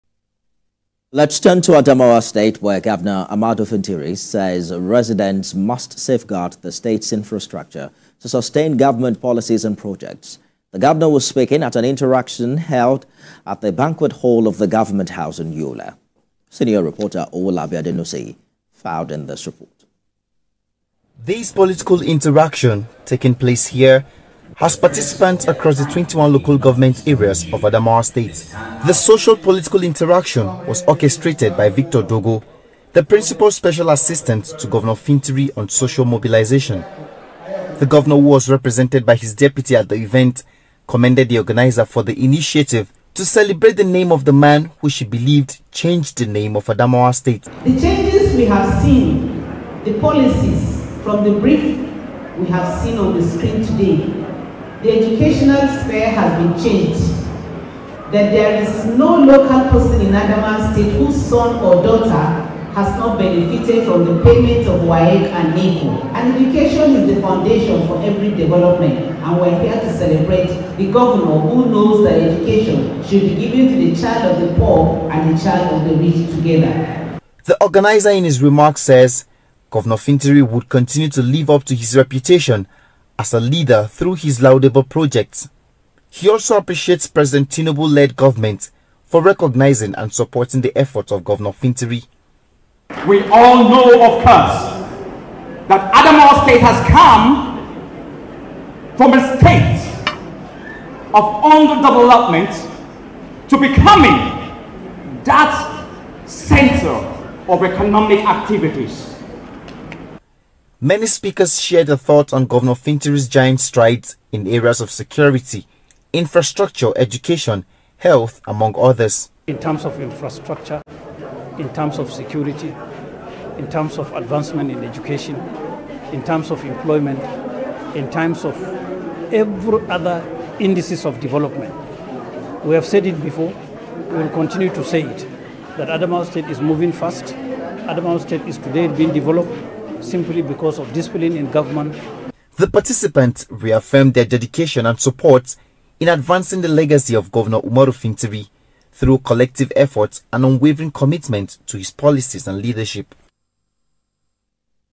Governor Fintiri Asks Residents To Protect Government Properties In Adamawa
Governor Ahmadu Fintiri says residents must safeguard the state’s infrastructure to sustain government policies and projects.
The Governor was speaking at an interaction held at the banquet hall of the government house in Yola.